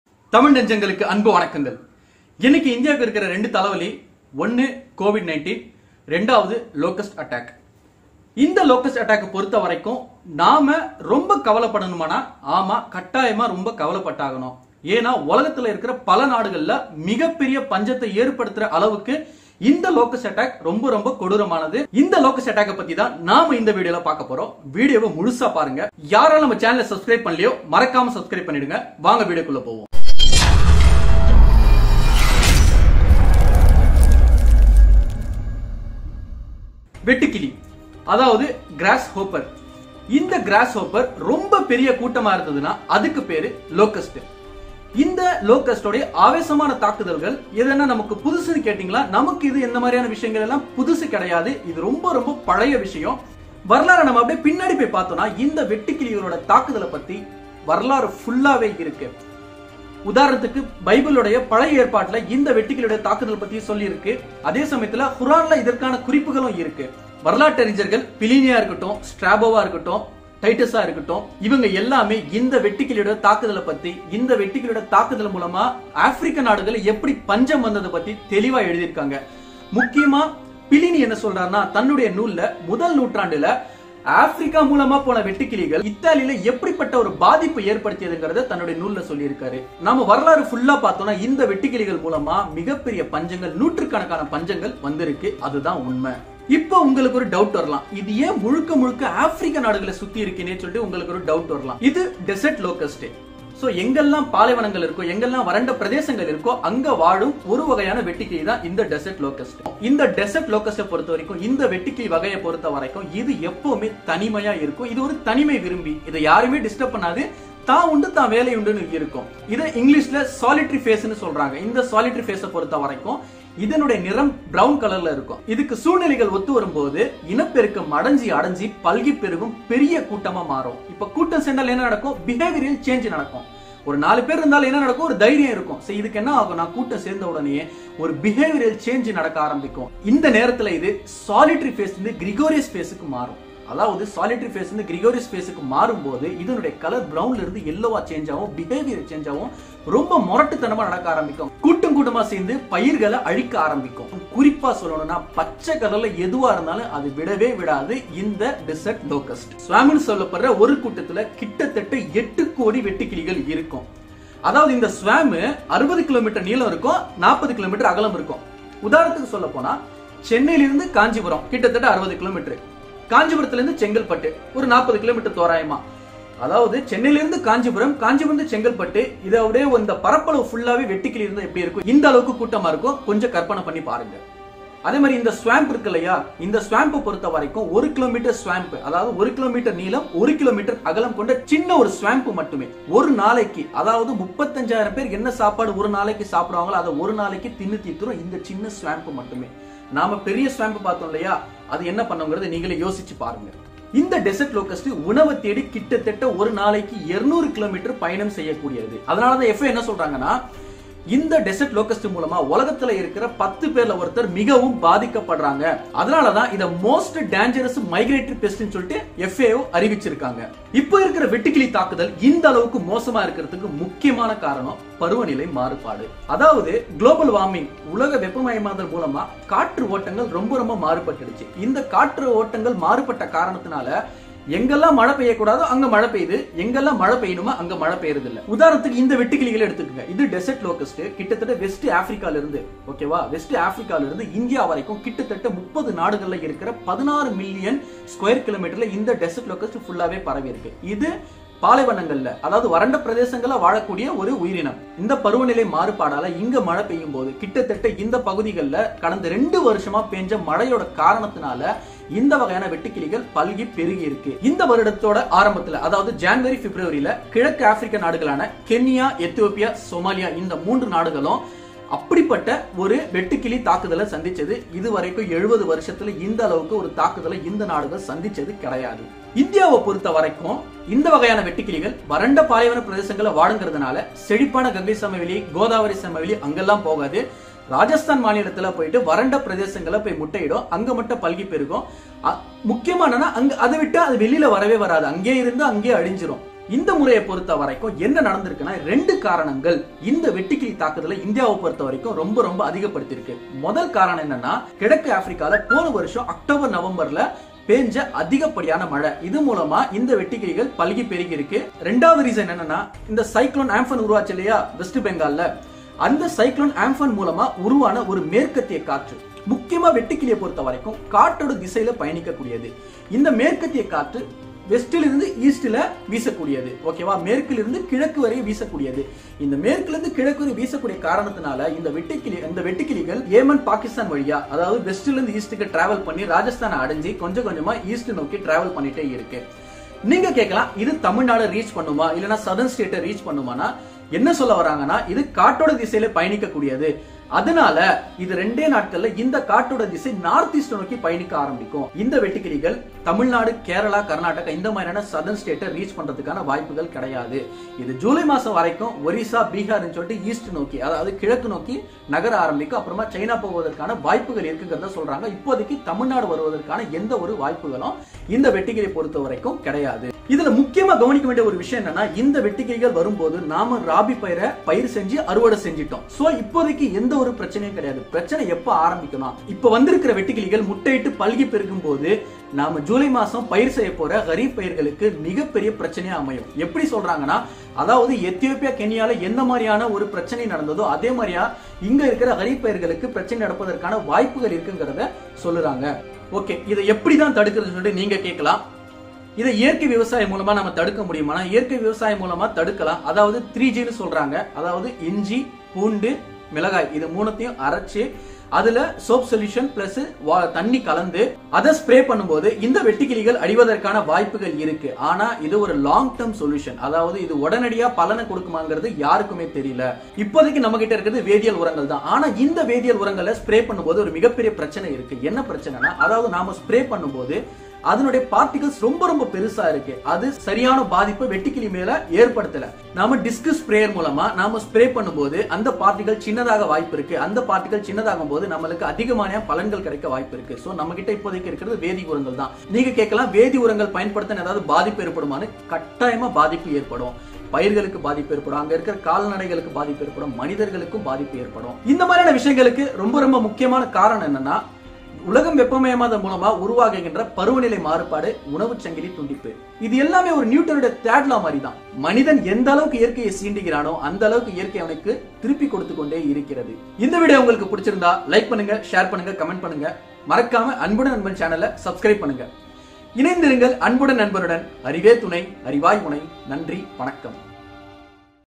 Is Locust attack normal? | Will it affect Tamilnadu?
Locusts are a collection of certain species of short-horned grasshoppers in the family Acrididae that have a swarming phase. These insects are usually solitary, but under certain circumstances they become more abundant and change their behaviour and habits, becoming gregarious